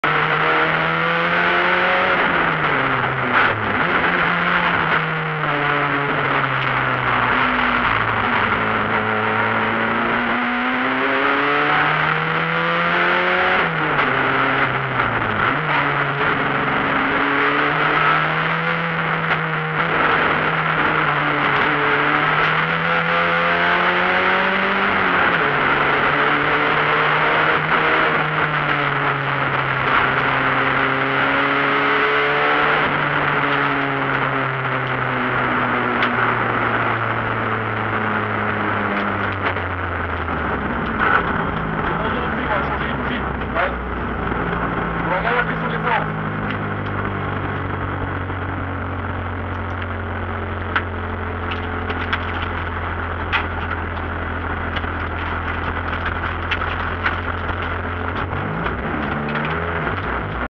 montee historique chamrousse 2016 n3 r21 europa cup
montee historique chamrousse 2016 l'aprés midi le brouillard c'est levé nickel